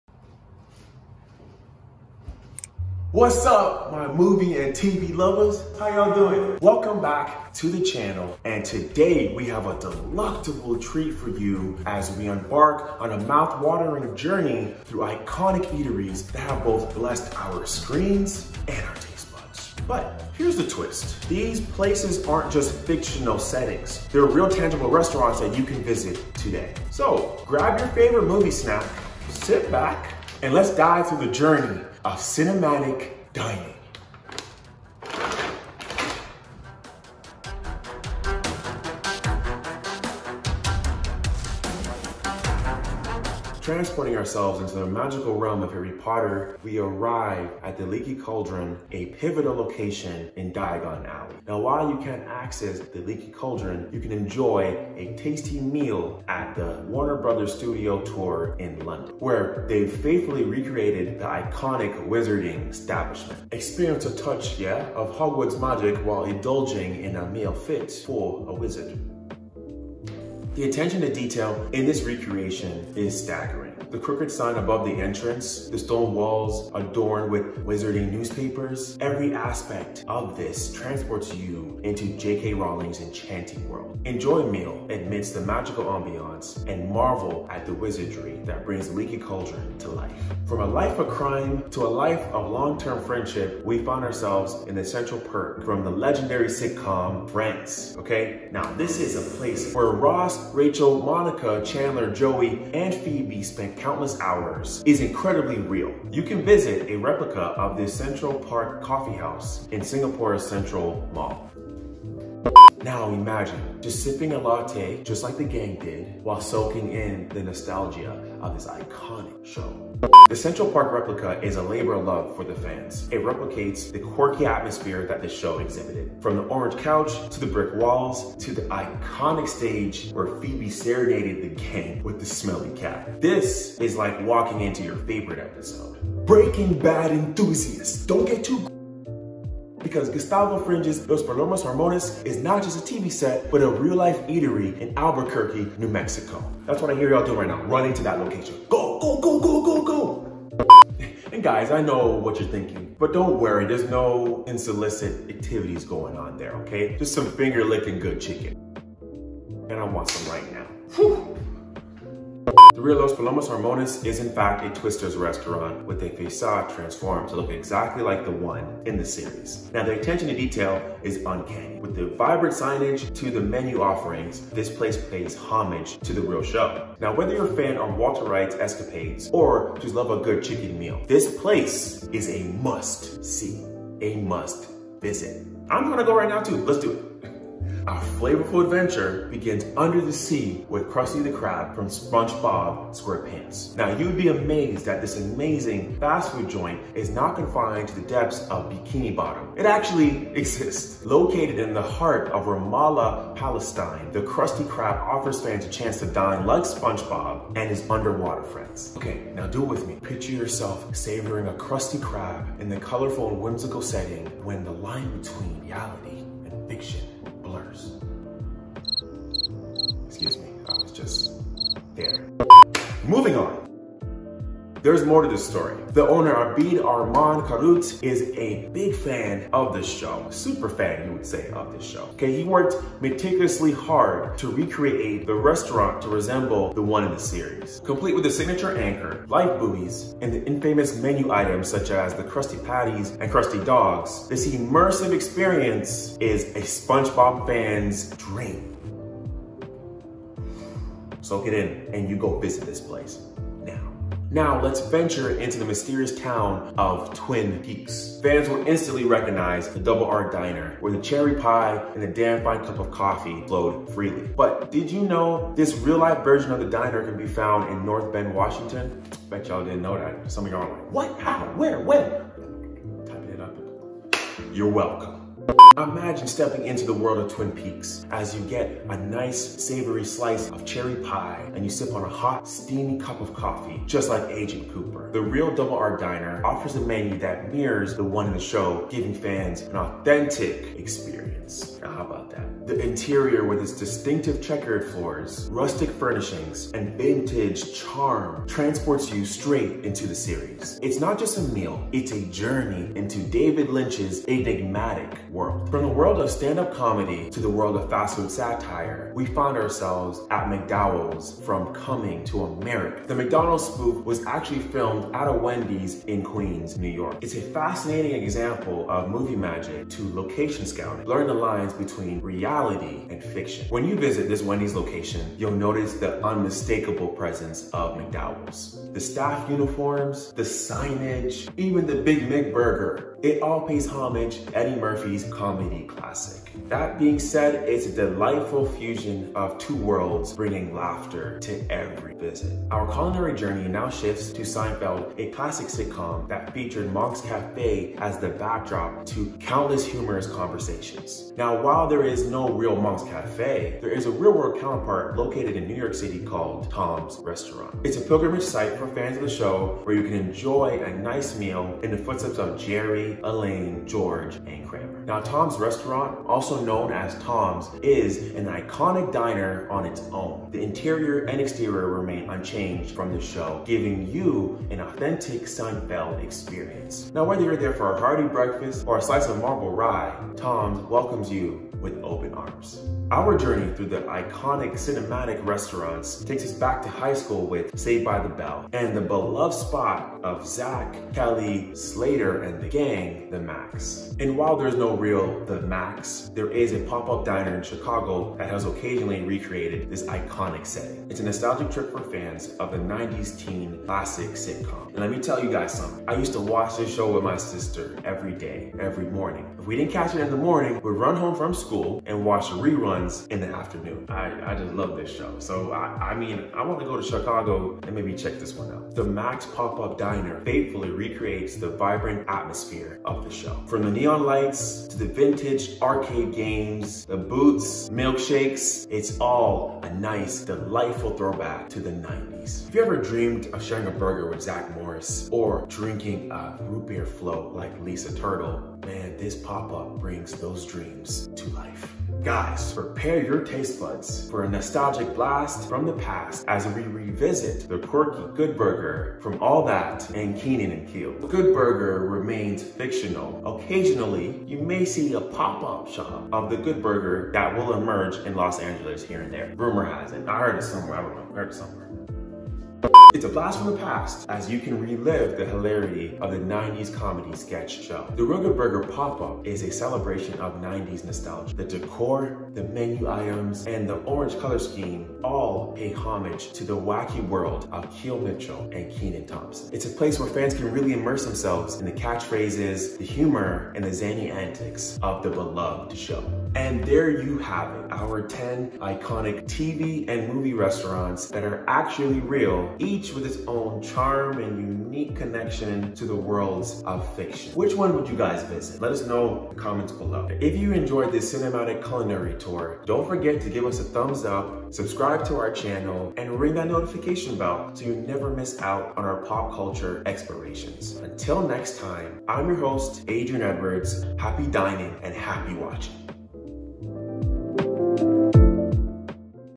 Top 10 Craziest Restaurants That Actually Exist
People who really like TV shows and movies often want to go to the same places they see in their favorite shows. They especially like going to restaurants where the characters in the show eat the same food. Some of these restaurants were where they actually filmed the show, and others just gave the idea for the pretend restaurants. Here's a list of make-believe restaurants that many people like, and guess what? You can actually go to these real places!